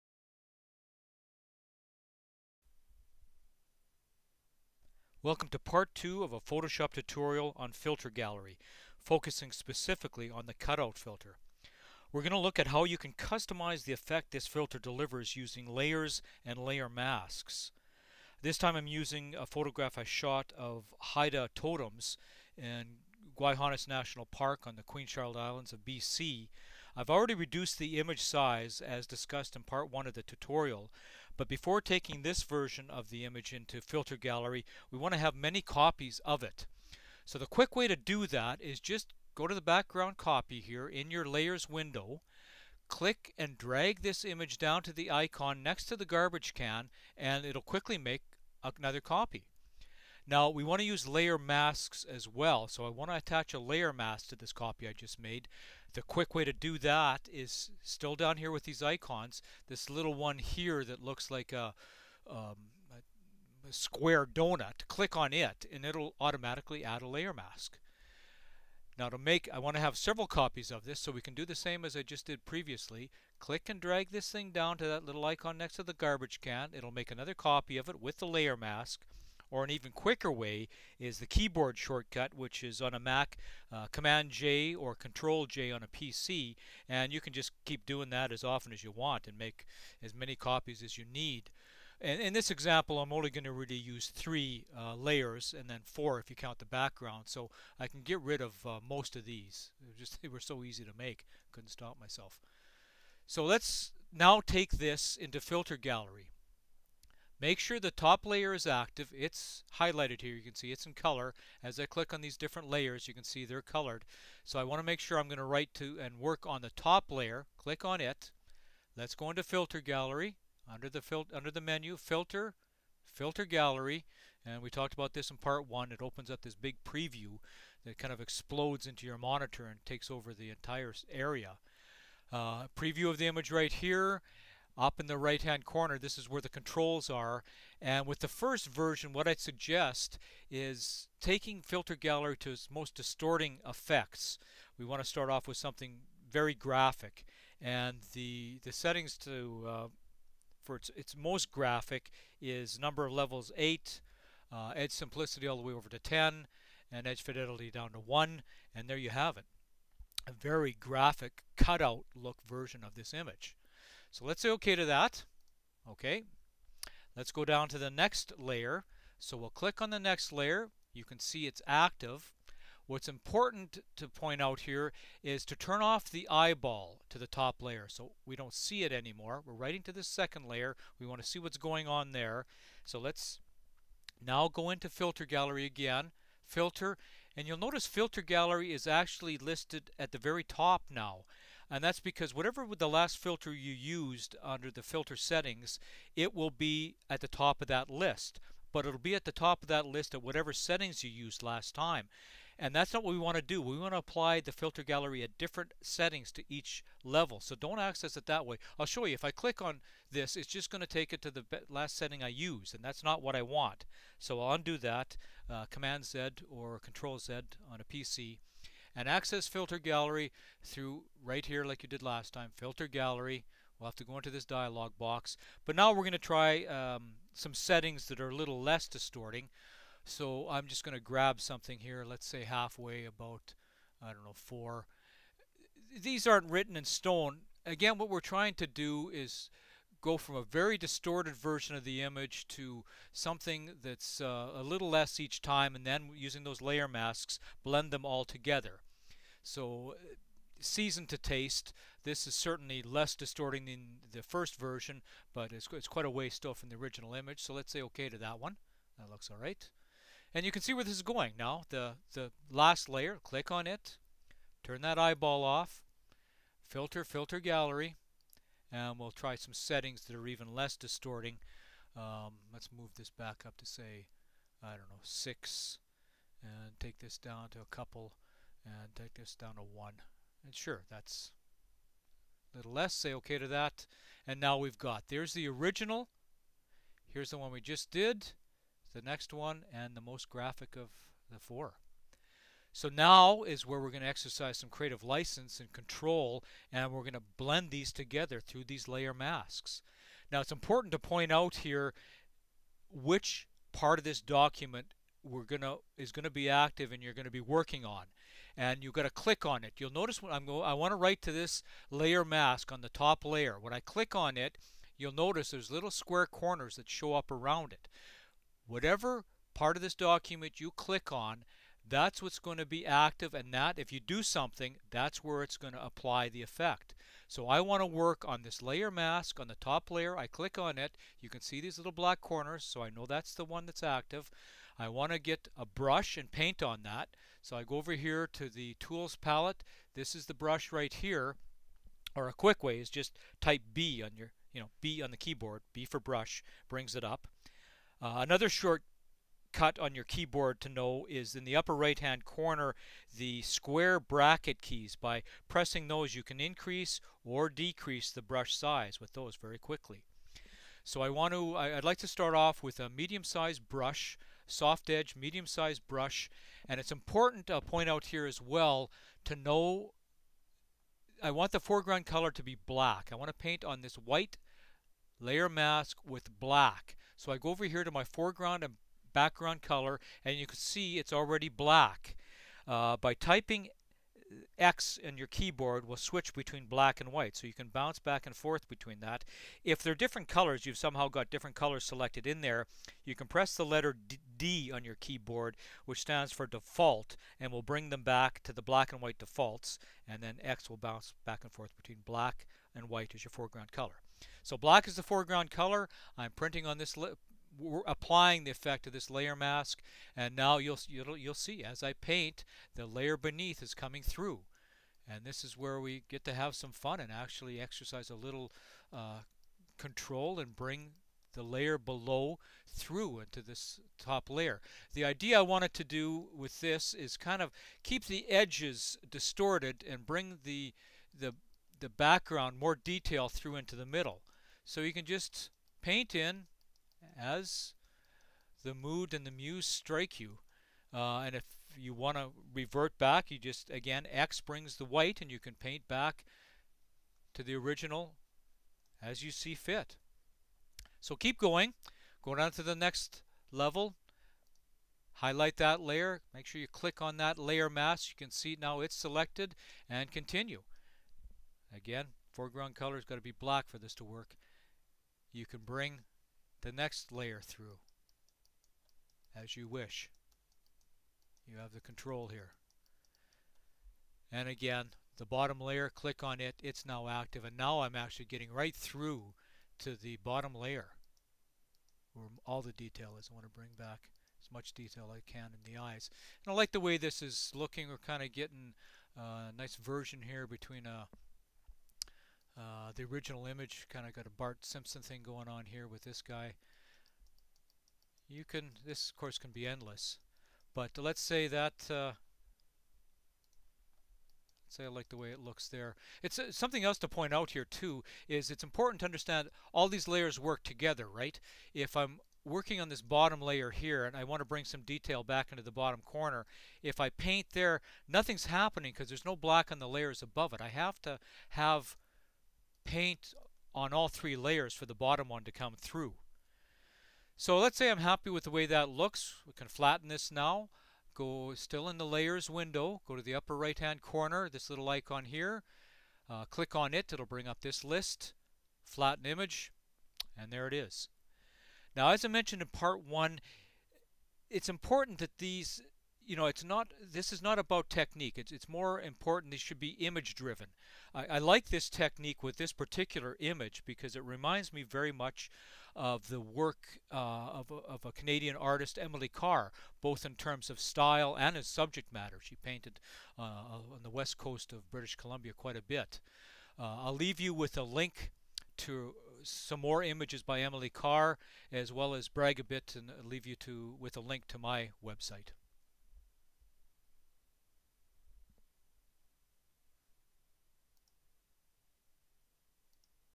Filter Gallery Part 2.mov
Part 2 tutorial on Photoshop's Filter Gallery, Cutout Filter, using layers and layer masks.